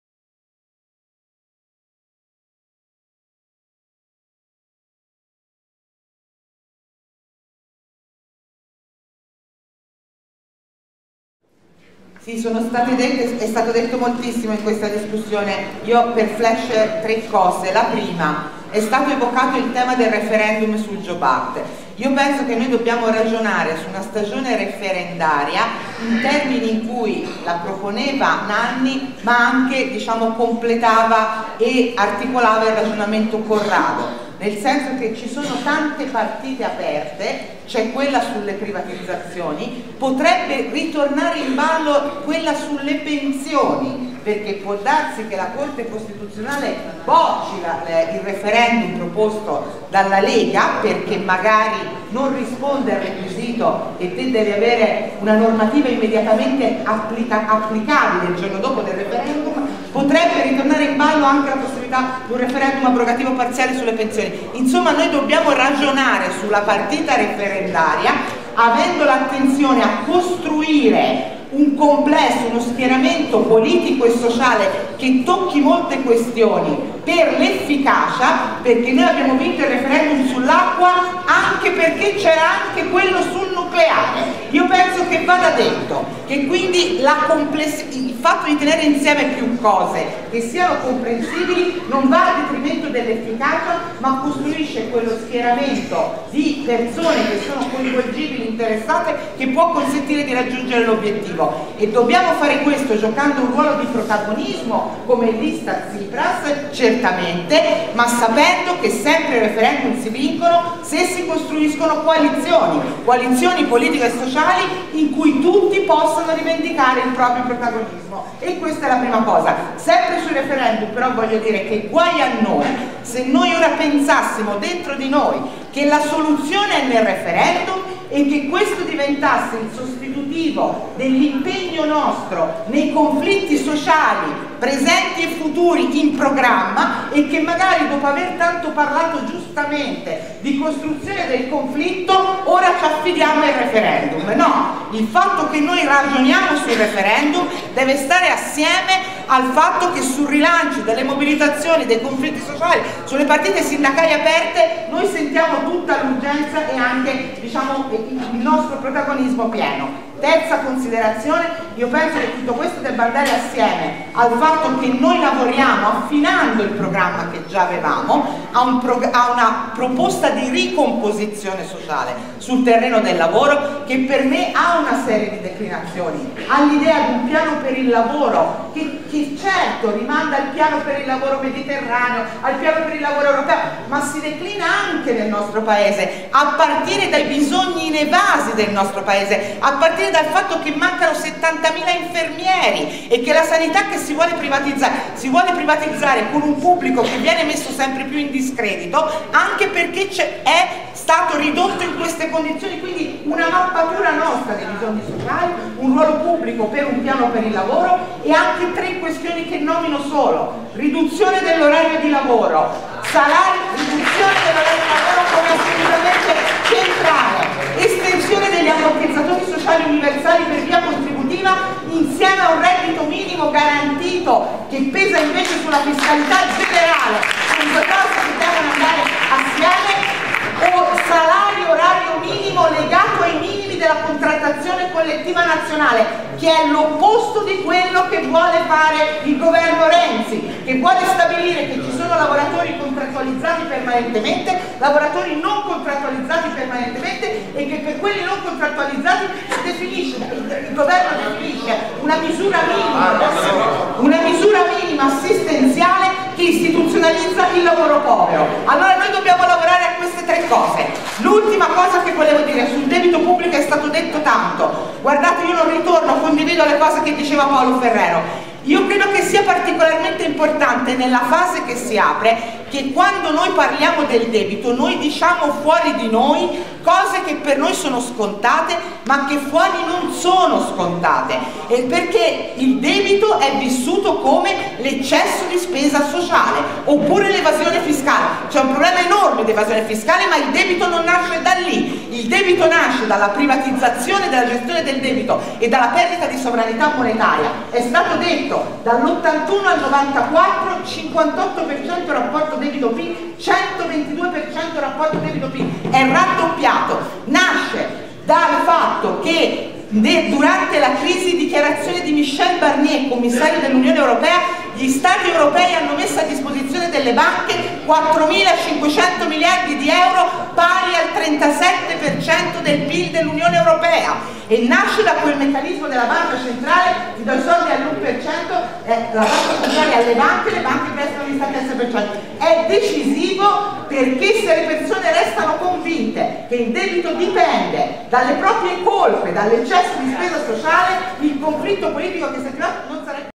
Roberta Fantozzi - I contenuti al centro assemblea nazionale l'Altra Europa
Intervento di Roberta Fantozzi - I contenuti al centro - Assemblea nazionale l'Altra Europa con Tsipras 18 Gennaio 2015 Bologna